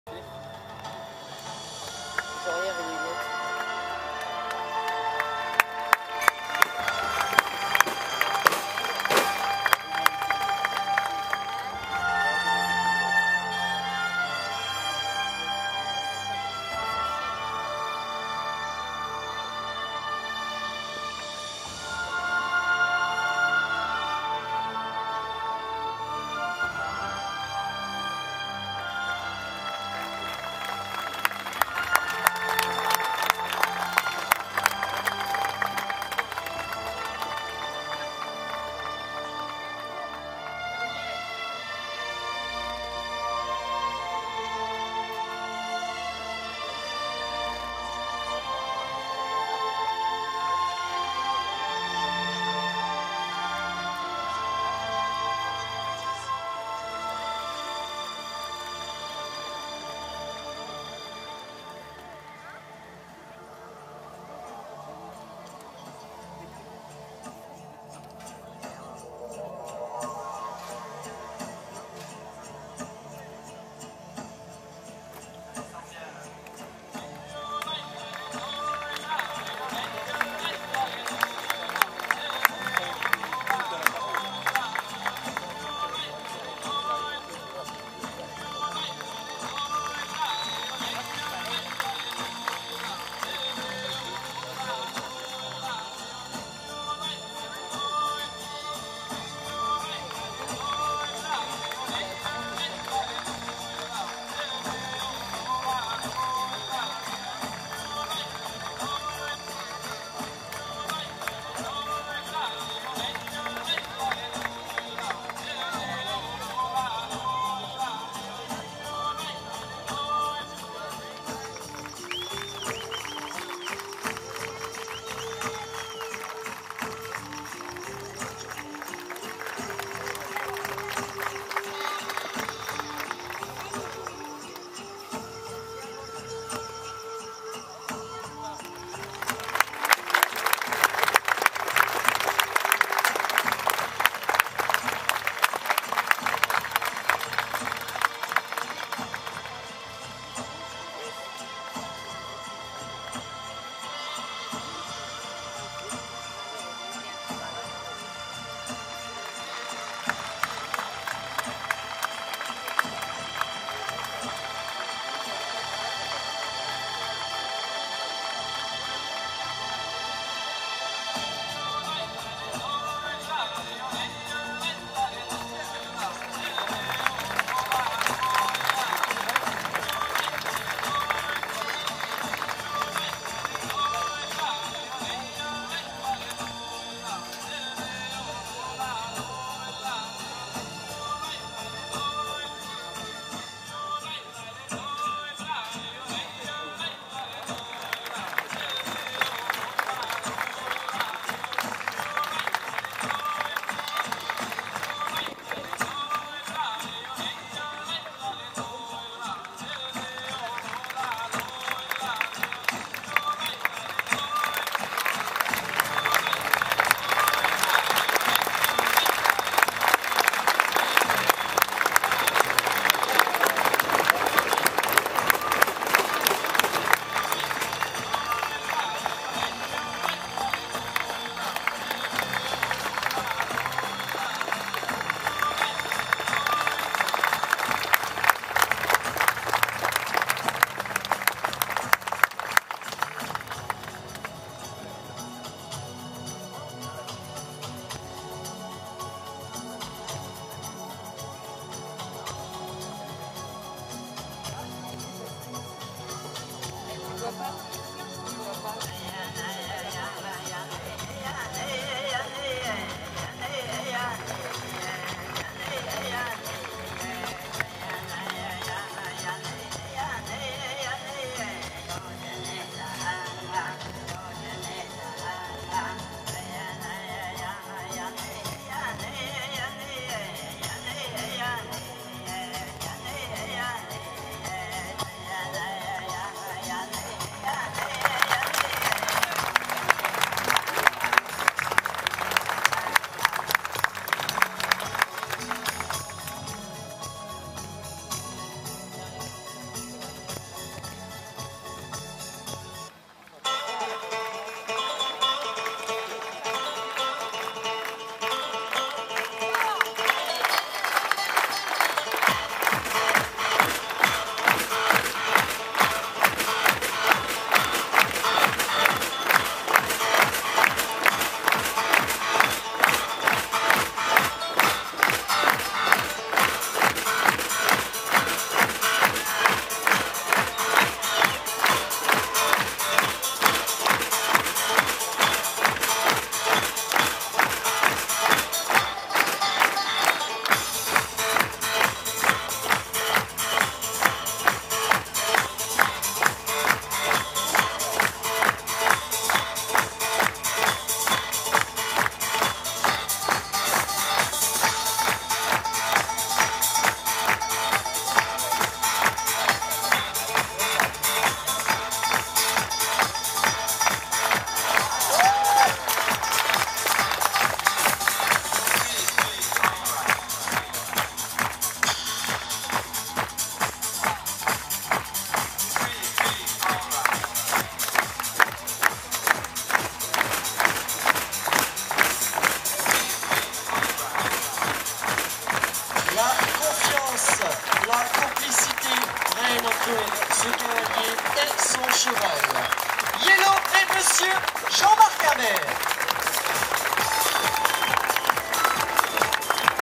West country 2012 bain de bretagne Jean Marc Imbert
Jean Marc Imbert dans son numéro de l'indien au festival West country 2012 de bain de bretagne